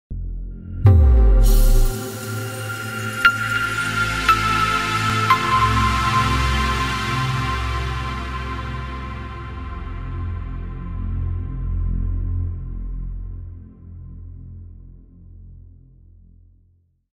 GoodSoundForYou Intro
My new Video Intro made by  TheUltimateFX
Sounddesign by myself.